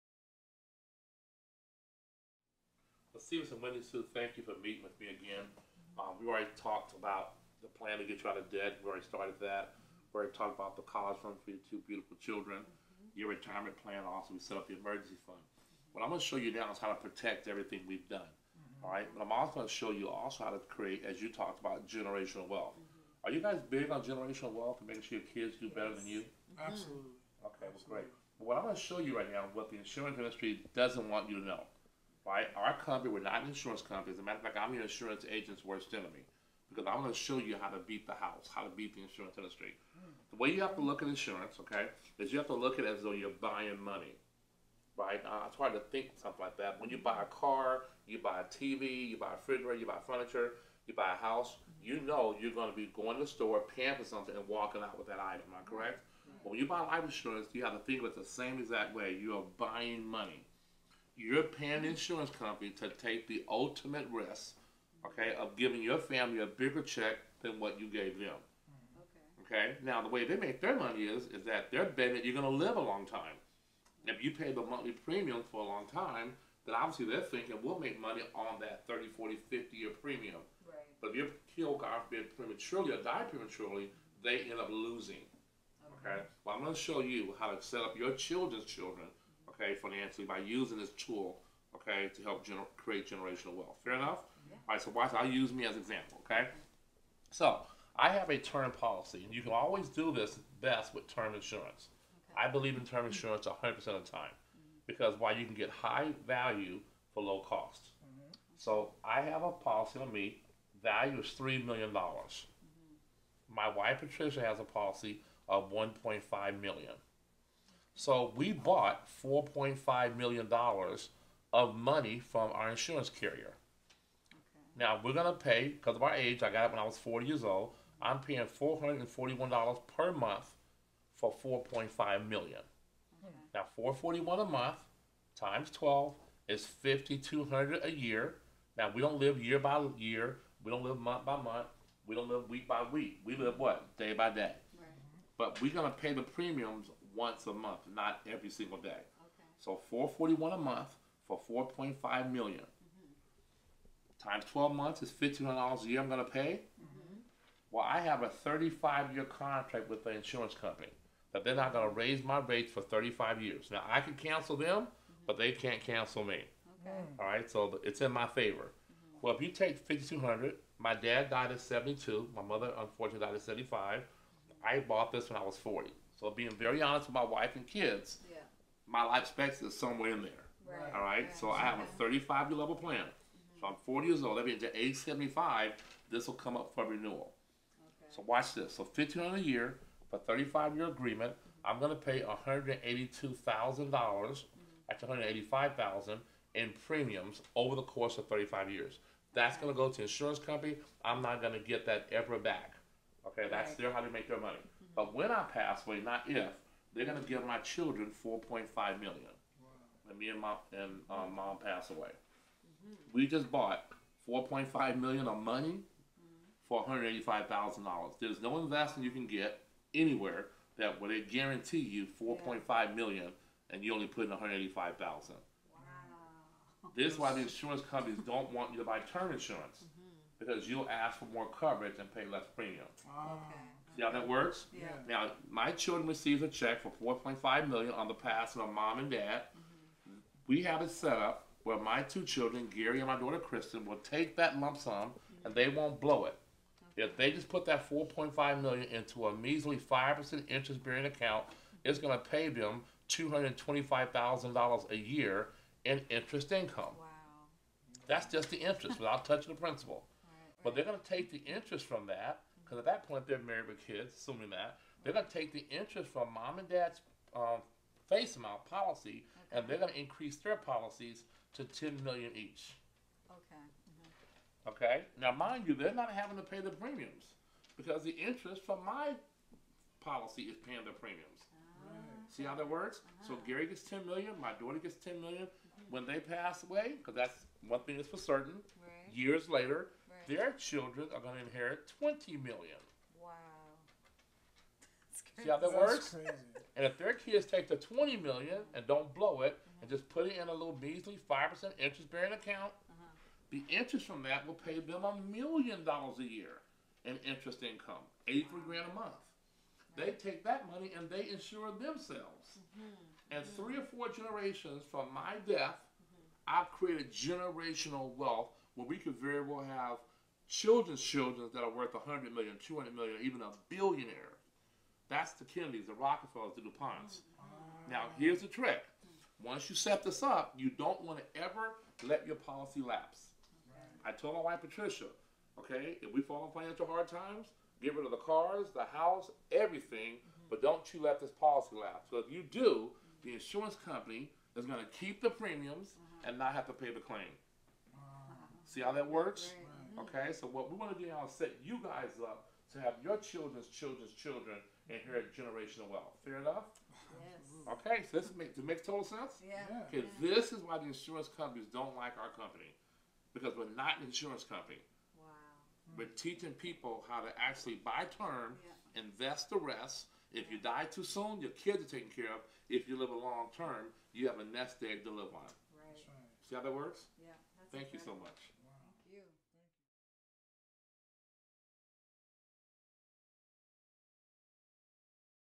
Generational Wealth
This strategy is as old as capitalism itself and has been the catalyst for creating and maintaining wealth for generations. Any financially savoy person who has forward thinking will take advantage of this strategy.